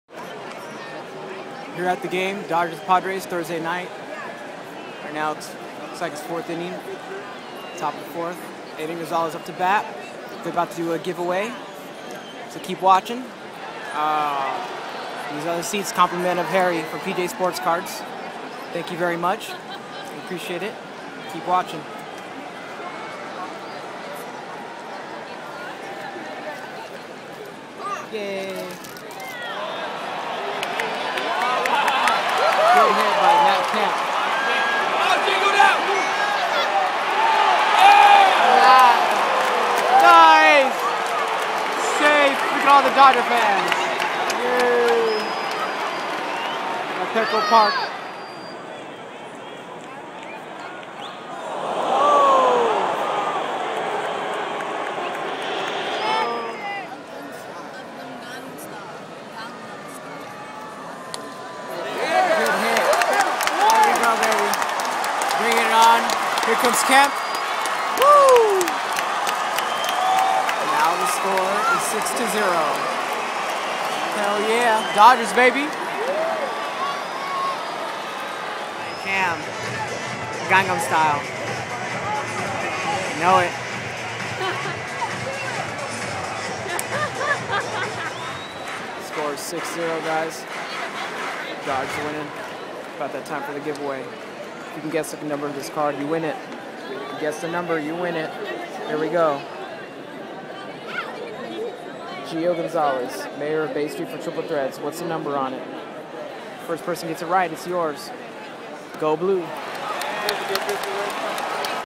AT THE BALLGAME, GIVEAWAY EDITION (WE HAVE A WINNER - PUFFYBEAR5) !!!
Went To The Ballgame Last Night (Dodgers Vs. Padres) Compliments The Greatest Hobby Shop Owner Harry From PJ's Sports Cards In Chula Vista. I Saw My Dodgers Pull The Victory Over The Padres All From The First Row, It Was Amazing. Now Onto The Giveaway The First Person To Guess The Numbering On The Gio Gonzalez Triple Threads Jersey Insert Wins It.